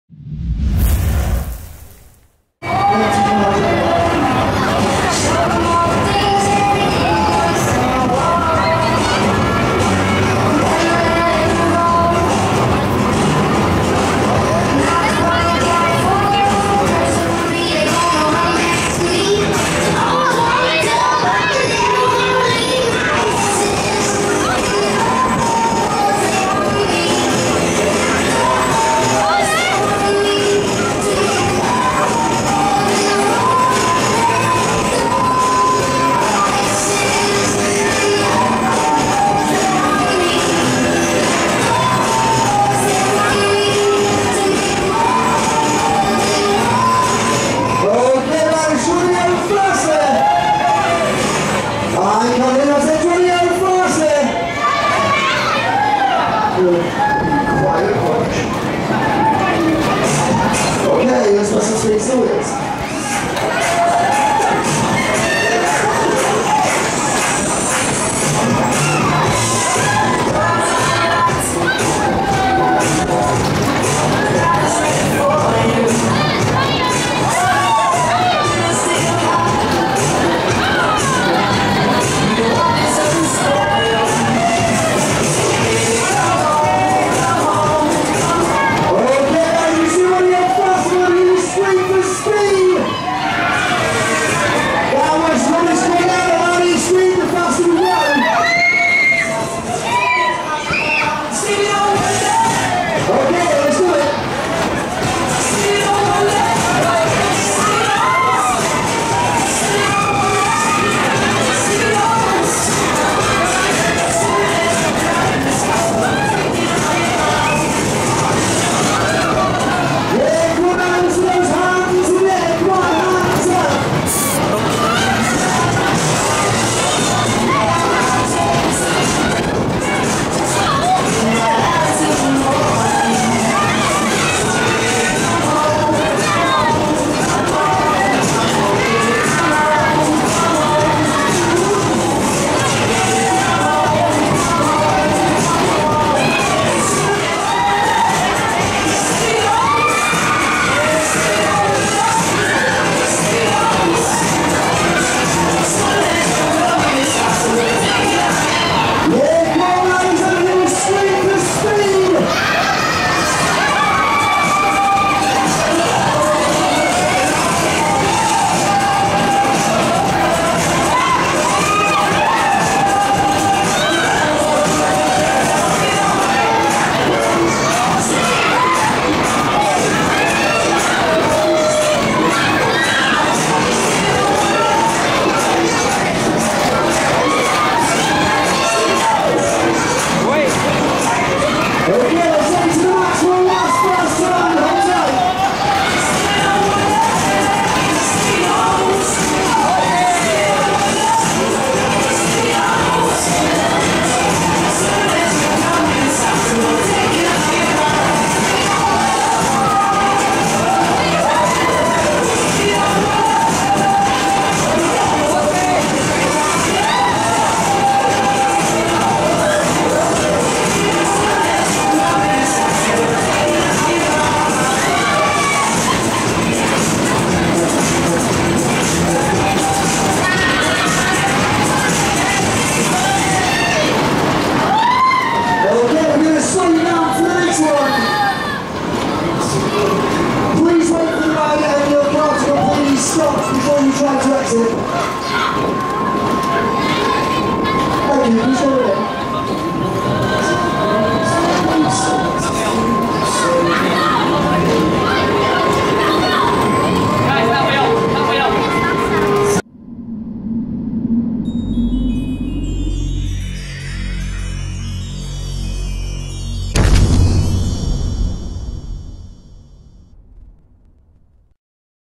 Blackpool South Pier - Hell-Raiser Offride
Offride footage of Hell-Raiser at the South Pier in Blackpool, Lancashire, UK.

This ride is a Maxwell Waltzer.